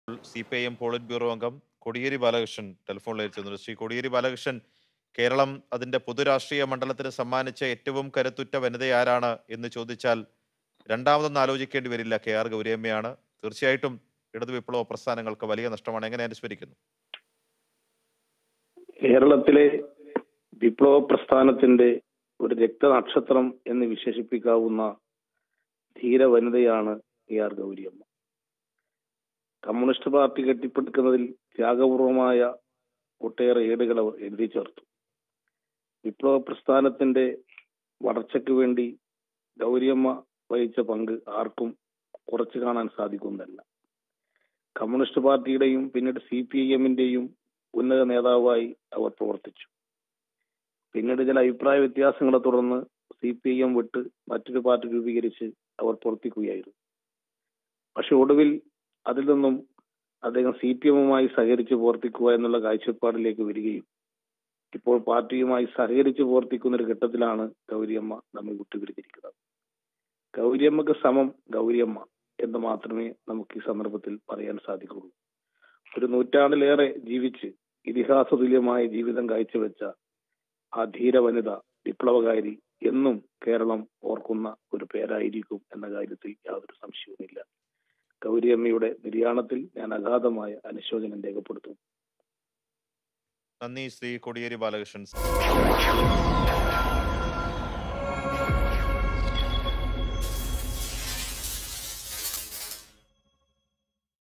ഗൗരിയമ്മയ്ക്ക് സമം ഗൗരിയമ്മ: കോടിയേരി| Kodiyari on Gowriamma
Kodiyeri remembering KR Gowriammal ഗൗരിയമ്മയ്ക്ക് തുല്യം ഗൗരിയമ്മ മാത്രമേ ഉള്ളൂവെന്ന് കോടിയേരി ബാലകൃഷ്ണൻ